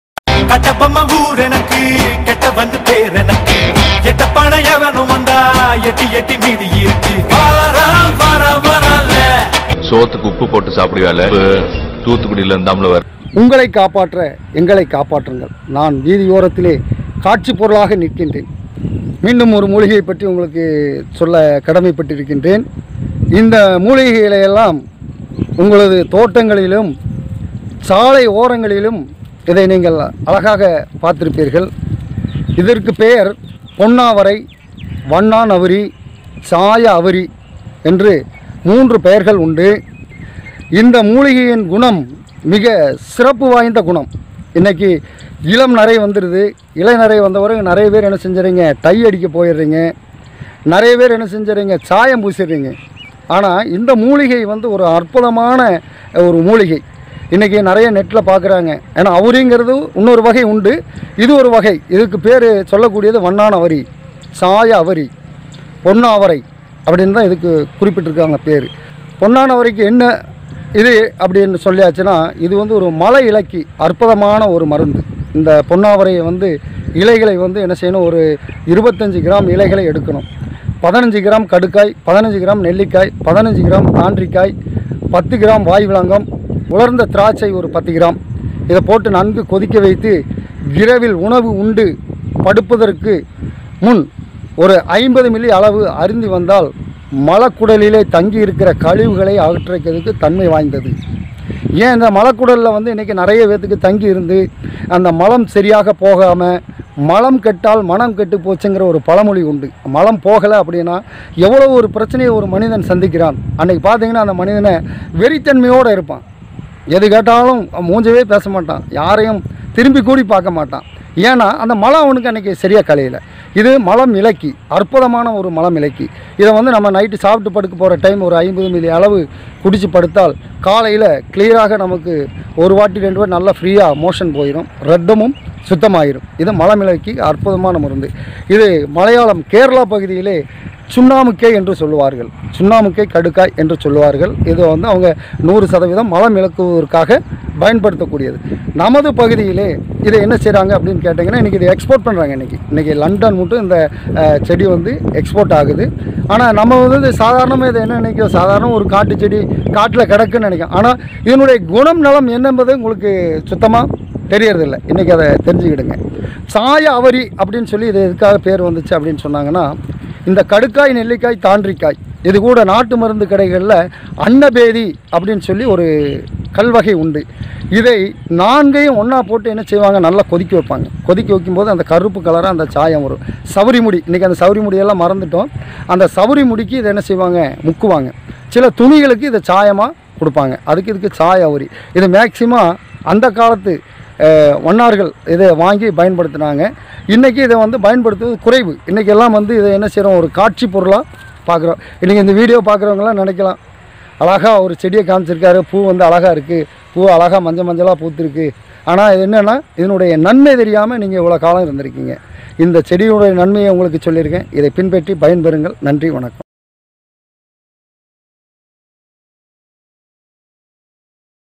வெள்ளை முடிக்கு சித்த மருத்துவரின் அறிவுரைகள் | Dr.selvaraj | Tuty360
வெள்ளை முடி மற்றும் மலச்சிக்கலுக்கு மருத்துவர் கூறும் ஆலோசனைகள்

நமது Tuty360 சமூக வளைதளங்களுடன் இணைந்திருங்கள் எப்போதும்

Tuty360 website